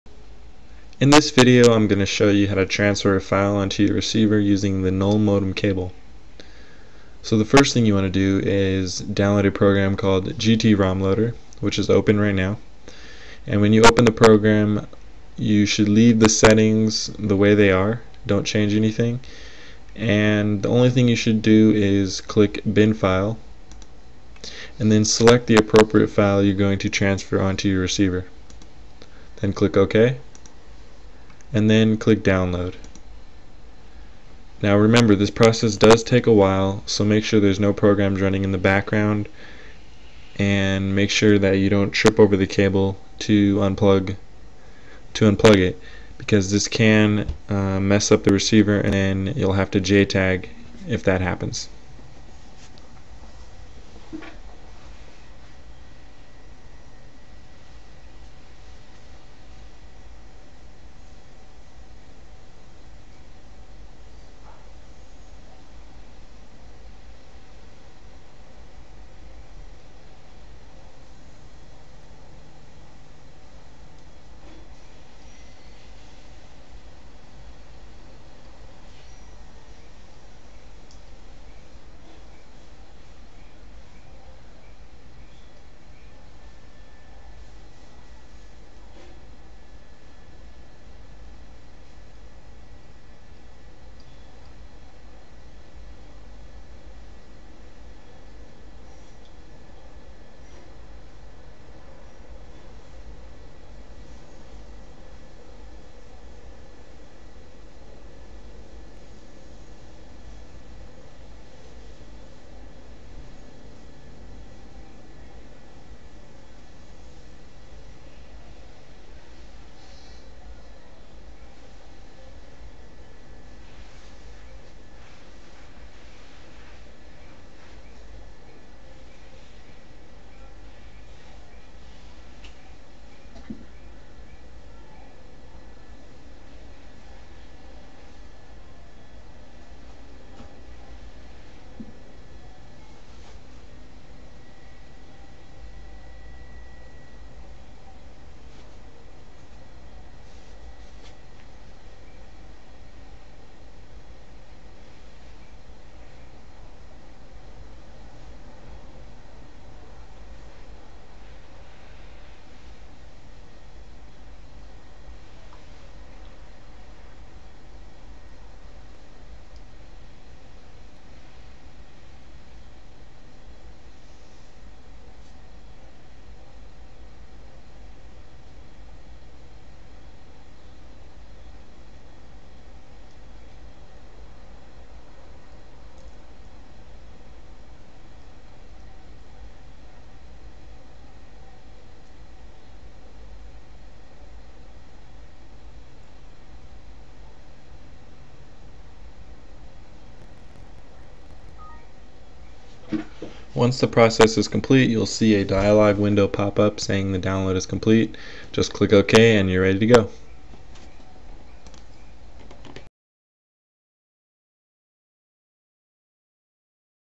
Pansat 3500S/SD [Flashing With Null Modem Cable]
PANSATSUPPORT.COM

Flashing the Pansat 3500S/SD receiver with the Null Modem Cable.